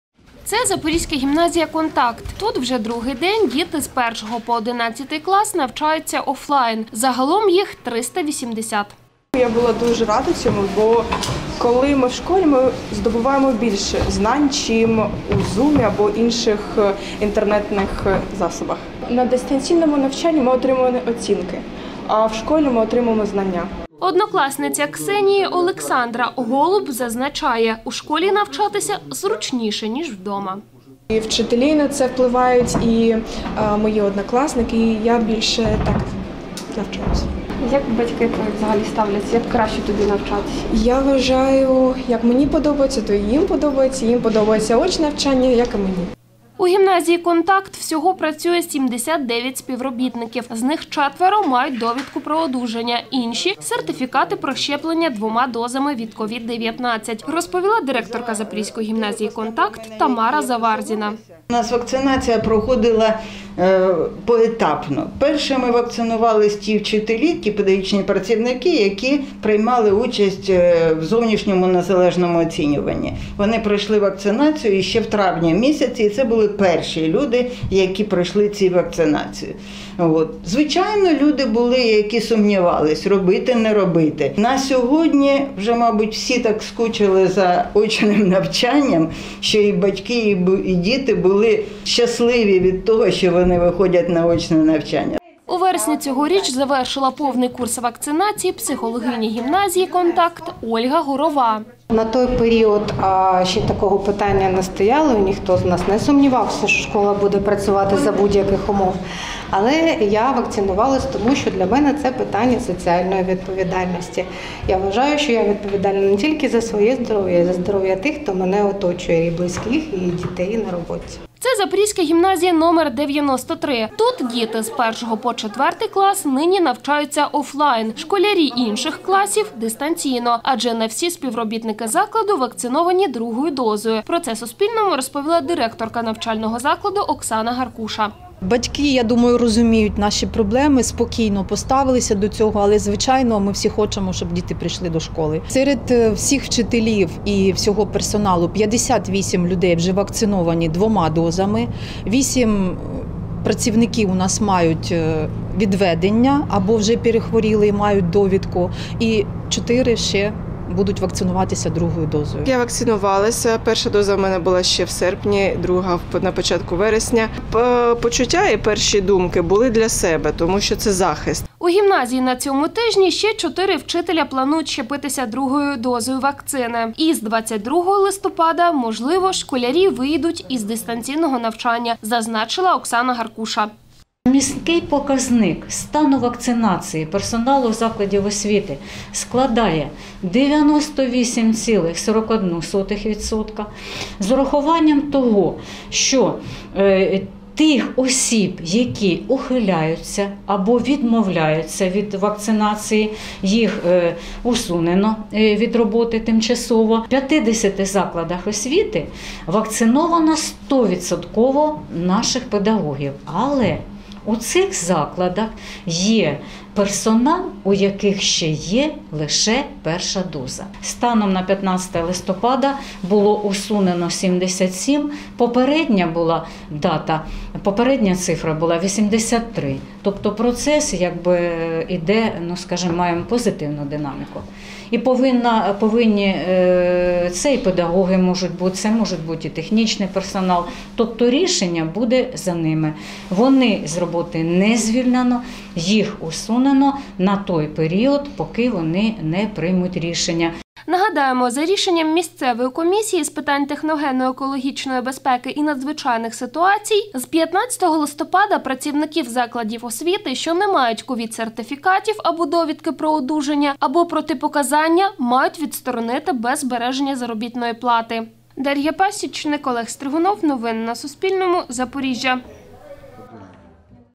У Запоріжжі заклади освіти почали працювати в офлайн-режимі | Новини
13 шкіл Запоріжжя станом на 16 листопада повністю працюють офлайн. Інші 105 - дистанційно, там відвідують школу тільки діти з 1-ого по 4 класи. Про це Суспільному повідомила заступниця директора департаменту освіти і науки Запорізької міської ради Наталія Вітковська. Як нині відбувається навчальний процес у школах, де вакциновано 100% персоналу та як навчаються школярі закладів, де ще не досягли таких показників.